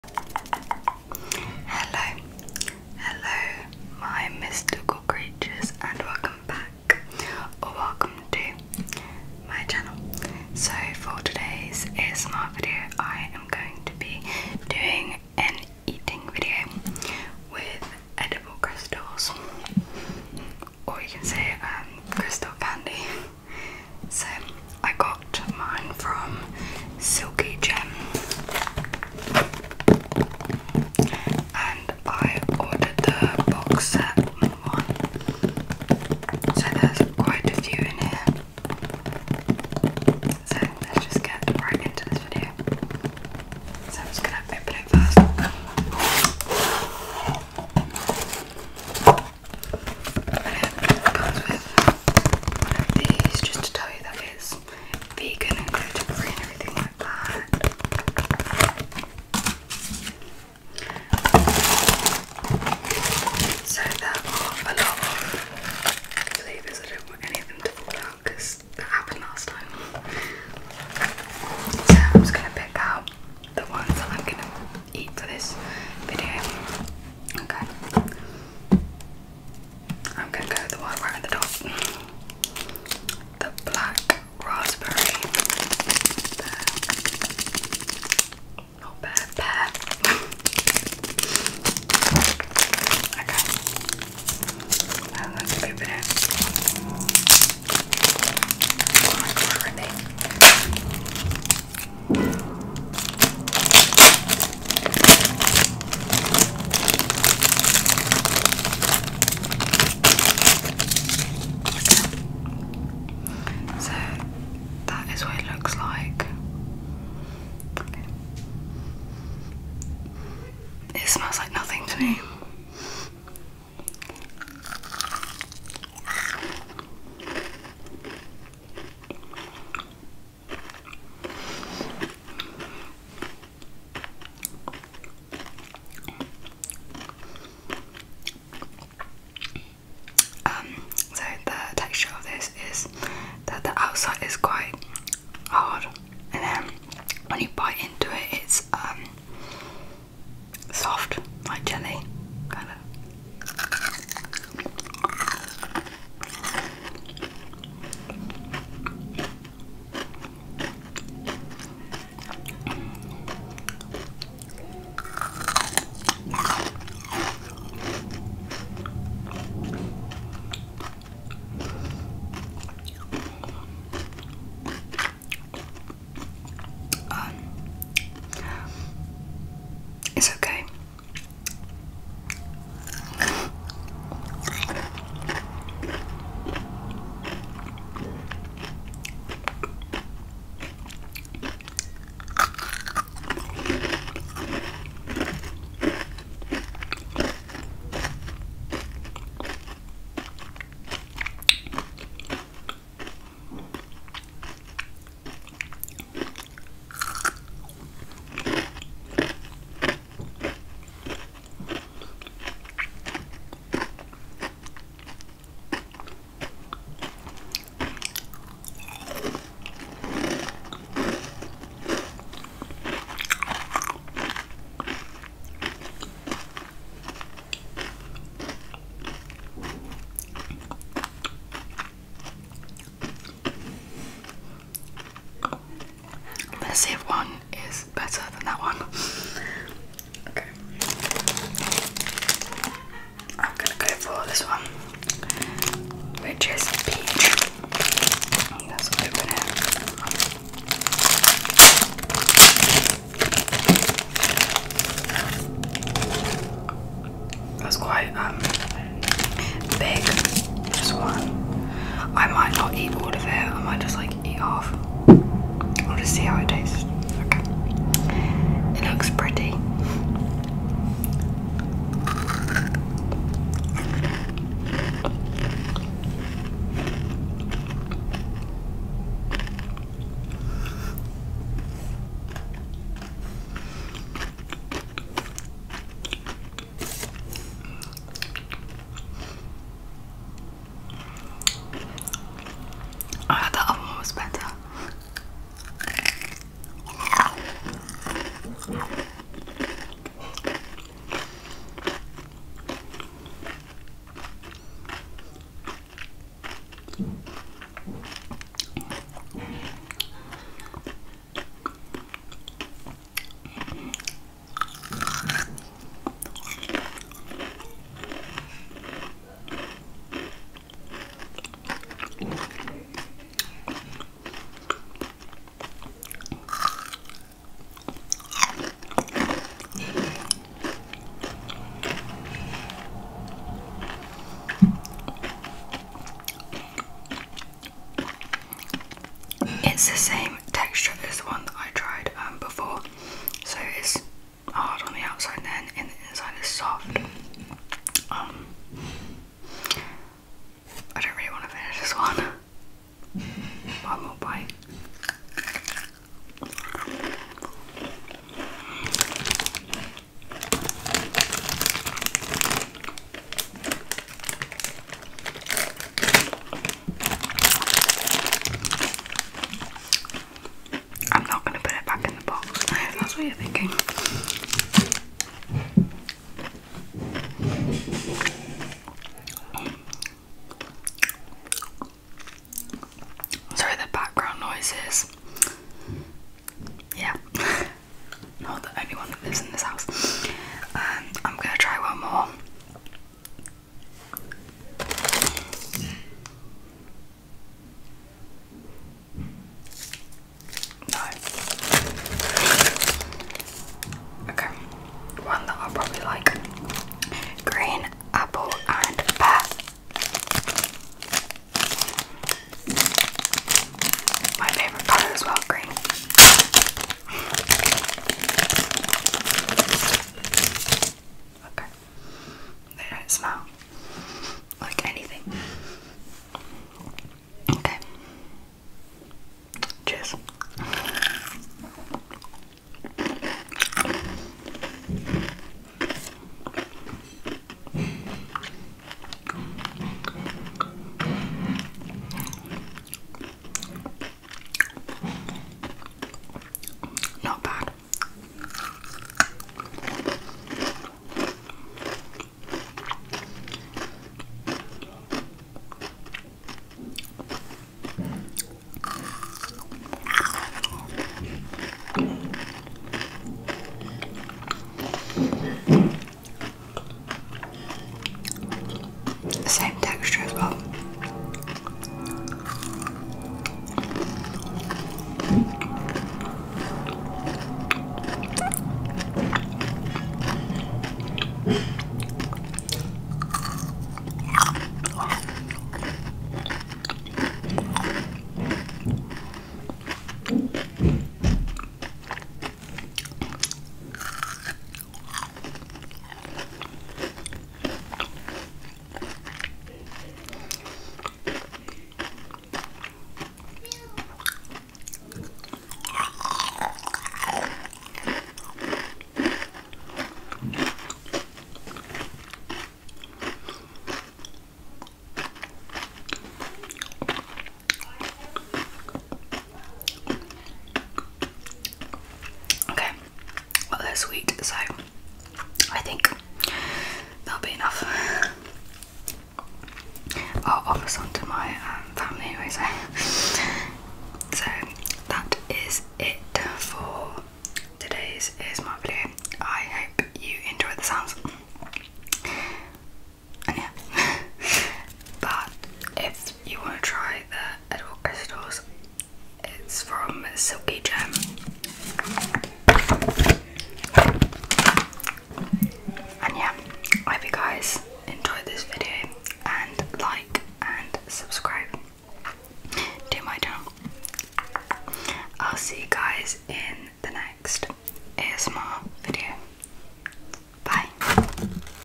ASMR| Eating Crystal Candy 🍬| Silky Gem
Hello my mystical creatures, I hope you enjoyed today’s video😁

Check out their website if you wish to get edible crystals (crystal candy)